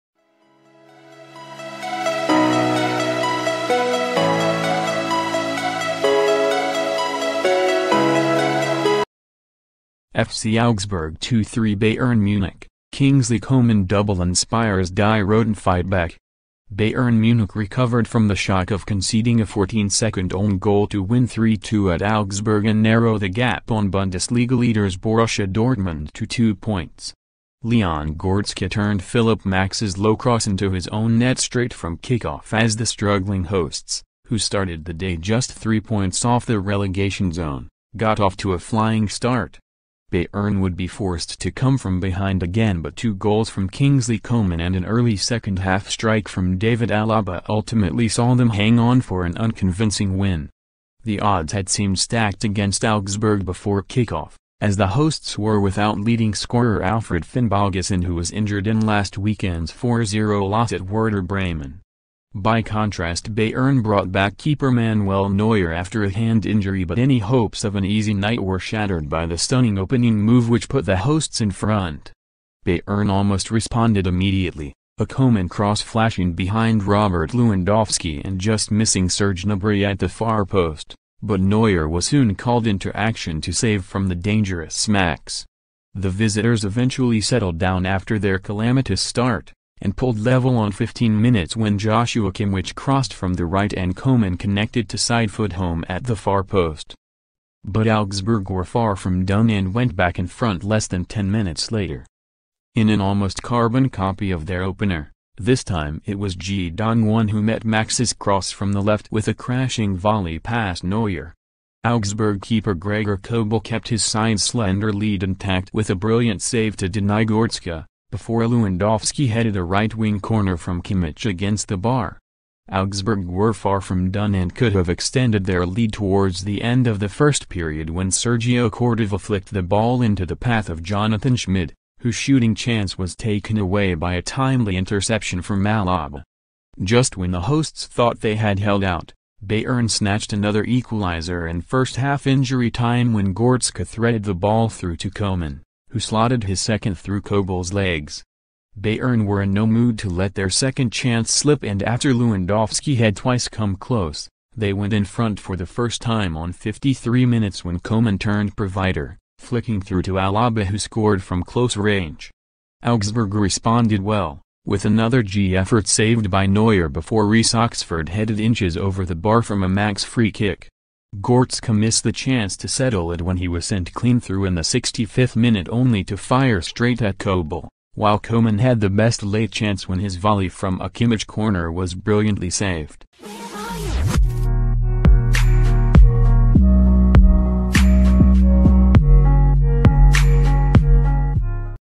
FC Augsburg 2 3 Bayern Munich Kingsley Coman double inspires Die Roten f ightback
FC Augsburg 2-3 Bayern Munich: Kingsley Coman double inspires Die Roten fightback. Bayern Munich recovered from the shock of conceding a 14-second own goal to win 3-2 at Augsburg and narrow the gap on Bundesliga leaders Borussia Dortmund to two points.

Leon Goretzka turned Philipp Max's low cross into his own net straight from kick-off as the struggling hosts, who started the day just three points off the relegation zone, got off to a flying start.

Bayern would be forced to come from behind again but two goals from Kingsley Coman and an early second-half strike from David Alaba ultimately saw them hang on for an unconvincing win.

The odds had seemed stacked against Augsburg before kick-off, as the hosts were without leading scorer Alfred Finnbogason who was injured in last weekend's 4-0 loss at Werder Bremen.

By contrast Bayern brought back keeper Manuel Neuer after a hand injury but any hopes of an easy night were shattered by the stunning opening move which put the hosts in front.

Bayern almost responded immediately, a Coman cross flashing behind Robert Lewandowski and just missing Serge Gnabry at the far post, but Neuer was soon called into action to save from the dangerous Max.

The visitors eventually settled down after their calamitous start, and pulled level on 15 minutes when Joshua Kimmich crossed from the right and Coman connected to sidefoot home at the far post.

But Augsburg were far from done and went back in front less than 10 minutes later. In an almost carbon copy of their opener, this time it was Ji Dong-won who met Max's cross from the left with a crashing volley past Neuer.

Augsburg keeper Gregor Kobel kept his side's slender lead intact with a brilliant save to deny Goretzka, before Lewandowski headed a right-wing corner from Kimmich against the bar.

Augsburg were far from done and could have extended their lead towards the end of the first period when Sergio Cordova flicked the ball into the path of Jonathan Schmid, whose shooting chance was taken away by a timely interception from Alaba.

Just when the hosts thought they had held out, Bayern snatched another equaliser in first-half injury time when Goretzka threaded the ball through to Coman, who slotted his second through Kobel's legs.

Bayern were in no mood to let their second chance slip and after Lewandowski had twice come close, they went in front for the first time on 53 minutes when Coman turned provider, flicking through to Alaba who scored from close range.

Augsburg responded well, with another Ji effort saved by Neuer before Reece Oxford headed inches over the bar from a Max free-kick.

Goretzka missed the chance to settle it when he was sent clean through in the 65th minute only to fire straight at Kobel, while Coman had the best late chance when his volley from a Kimmich corner was brilliantly saved.